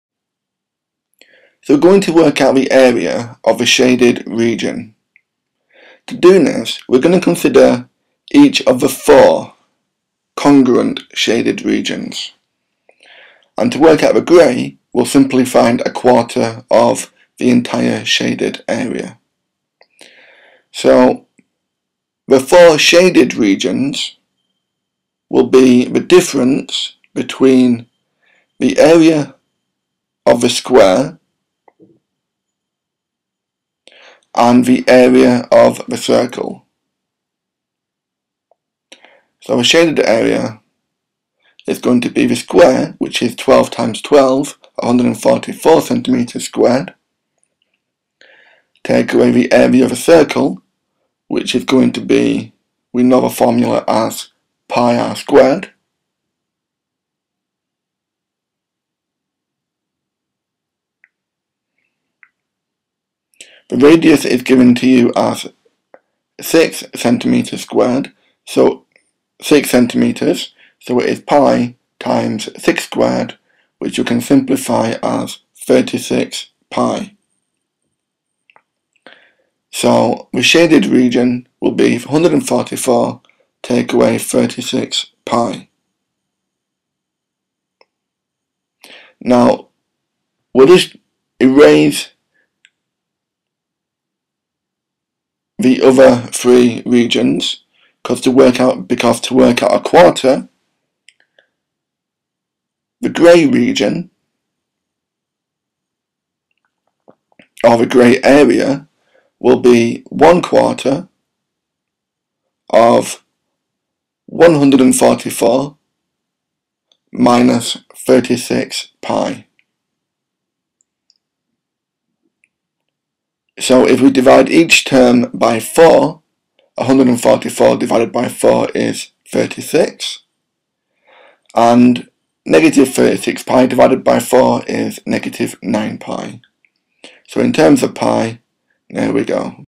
Calculating the Area of a Circle GCSE Mathematics Revision
GCSE Mathematics exam revision video that shows you how to calculate the area of circular shapes.  If you want further practise with this mathematics lesson or more mathematics resources in general then to download a printable worksheet with solutions.  Perfect revision for GCSE Mathematics!!